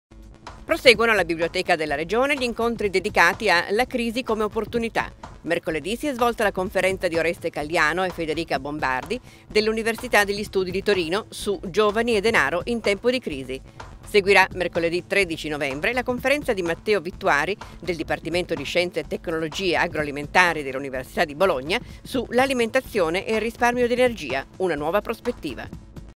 "Giovani e denaro in tempo di crisi"
Proseguono alla Biblioteca della Regione gli incontri dedicati a "La crisi come opportunità" con la conferenza di Oreste Calliano e Federica Bombardi, dell'Università degli Studi di Torino, su "Giovani e denaro in tempo di crisi". Seguirà, mercoledì 13 novembre, la conferenza di Matteo Vittuari del dipartimento di Scienze e tecnologie agro-alimentari dell'Università di Bologna su "L'alimentazione e il risparmio di energia: una nuova prospettiva".